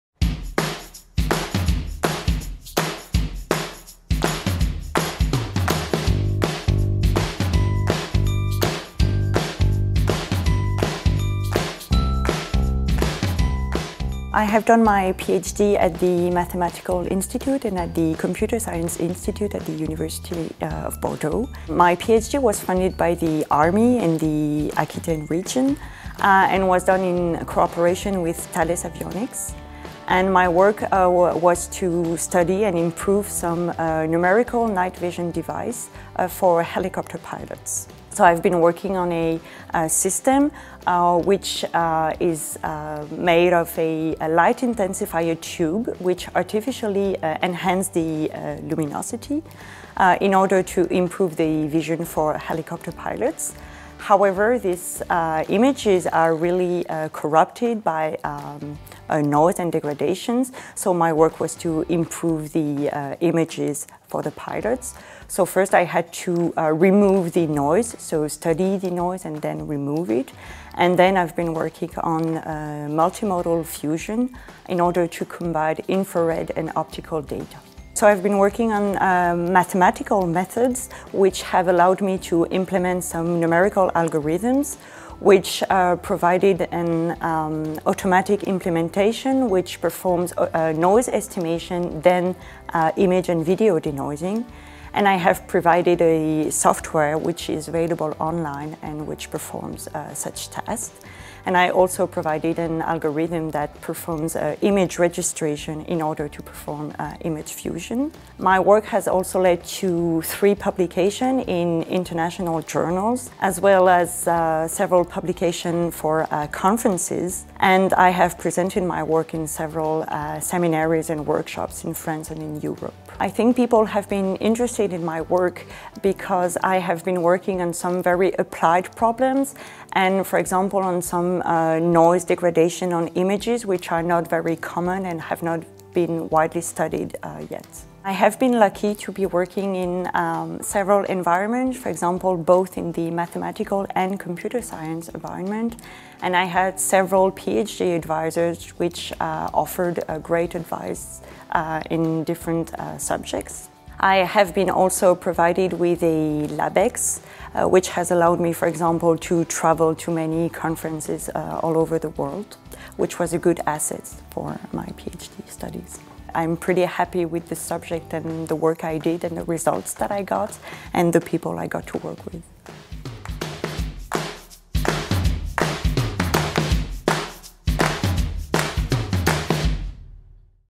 Prix de thèse 2016 « Sciences et technologies » - Camille Sutour
Prix « Sciences et technologies » à Camille Sutour (école doctorale Mathématiques et Informatique), pour ses travaux sur la « Vision nocturne numérique : Restauration automatique et recalage multimodal des images à bas niveau de lumière »